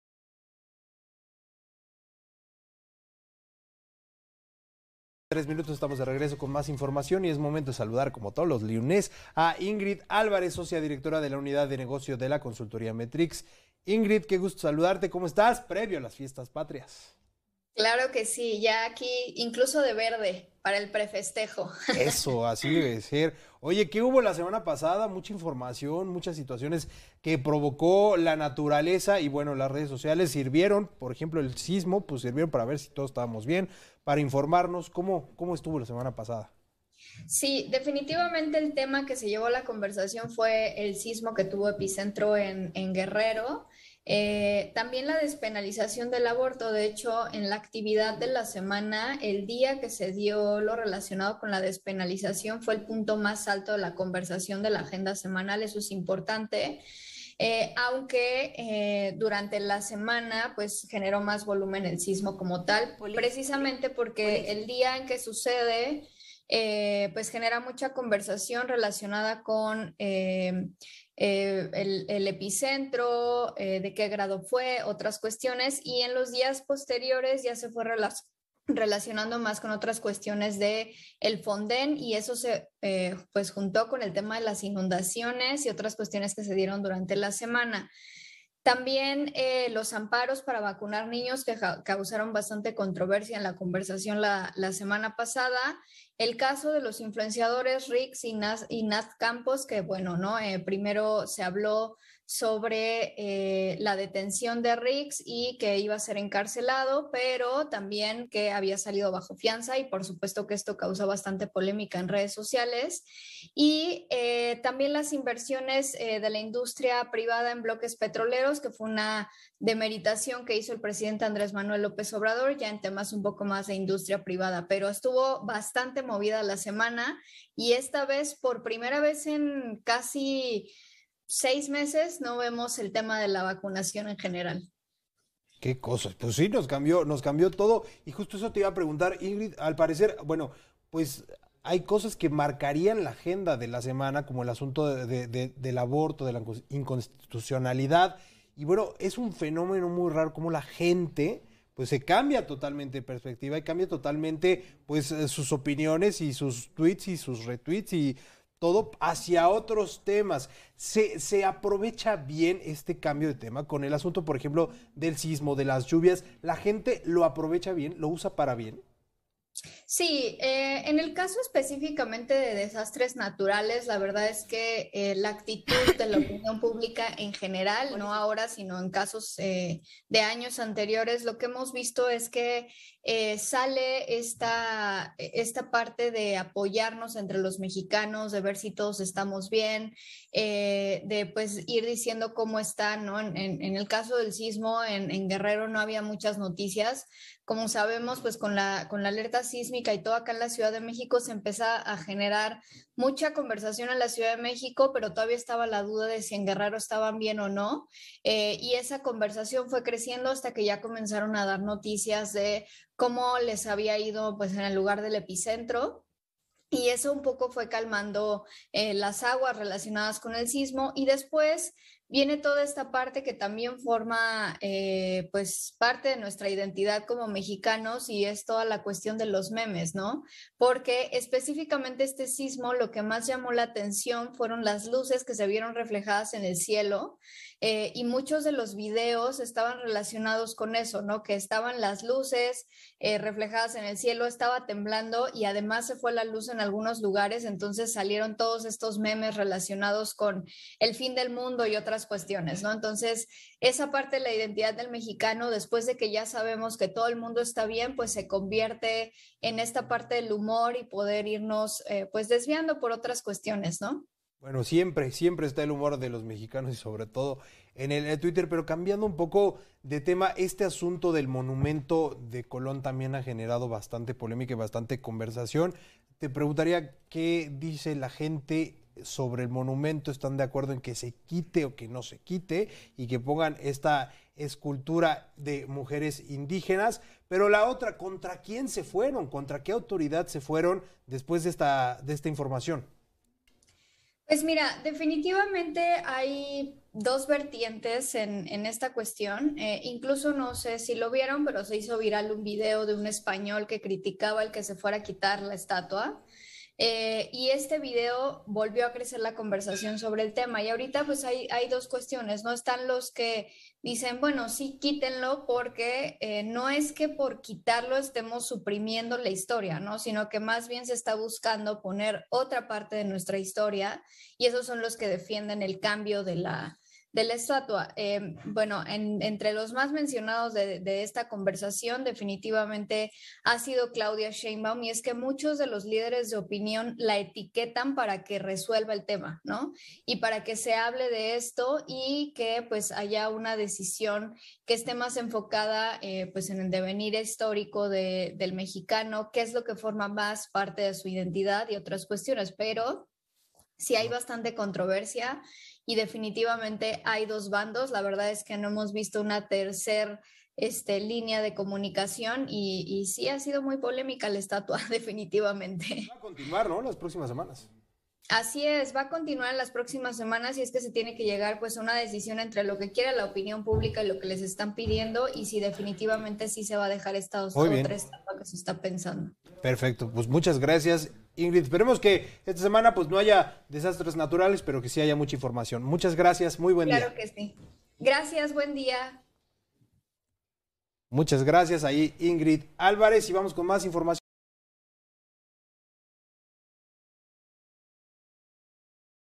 Sismo y despenalización del aborto, los temas más comentados en la Agenda Pública Digital.
Ingrid Álvarez nos cuenta sobre los temas más comentados en la Agenda Pública Digital del 4 al 10 de septiembre a través de AMX Noticias.